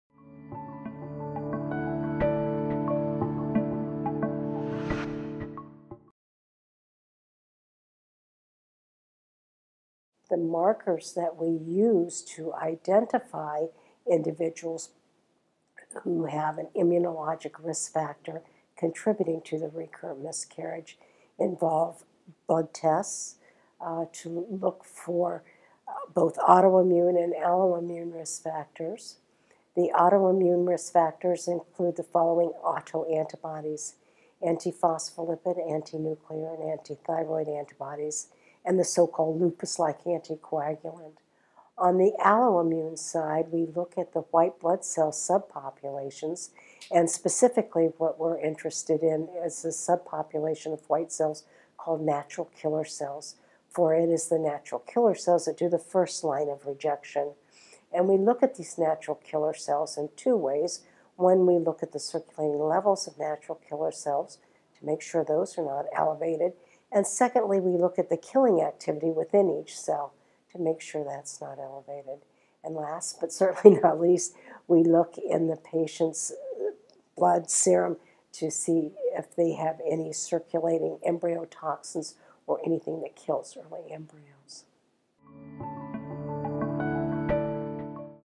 How are Immunological Causes of Recurrent Miscarriage Determined?
Carolyn B. Coulam, M.D., is an associate physician at Reproductive Medicine Institute and is Board Certified in the specialty of obstetrics and gynecology and in the subspecialty of reproductive endocrinology.